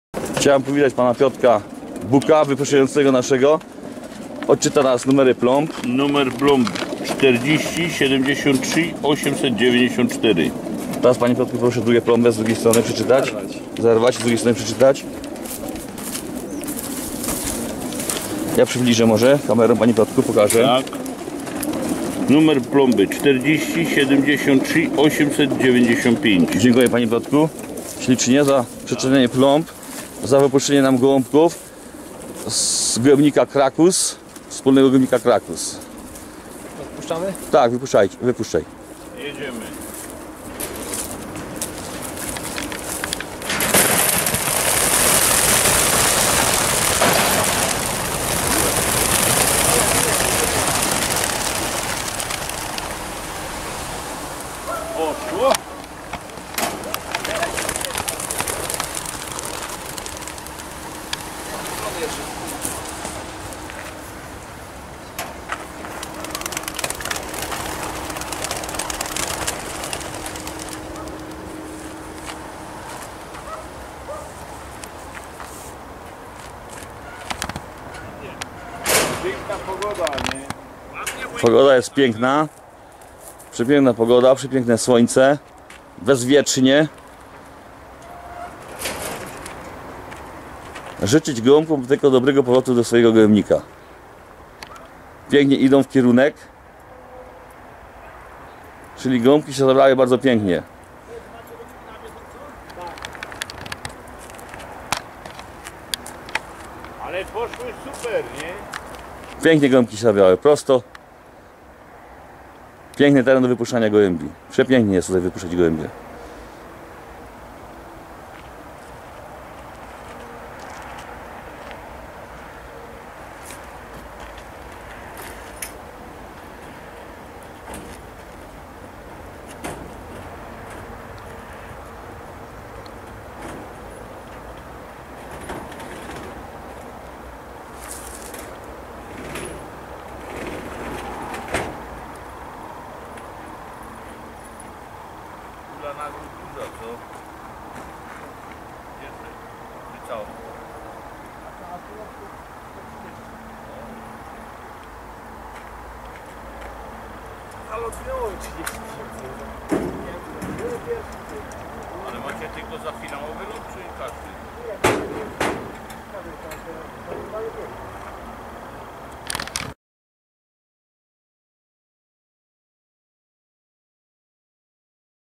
MWG KRAKUS 24.08.2015 - WYPUSZCZANIE GOŁĘBI/ TO FREE PIGEONS
WYPUSZCZANIE GOŁĘBI/ TO FREE PIGEONS

LOT KONKURSOWY NUMER 2/ RACE NUMBER 2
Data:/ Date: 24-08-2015
Z:/ from: Sośnicowice
Dystans:/ Distance: 155, 156 km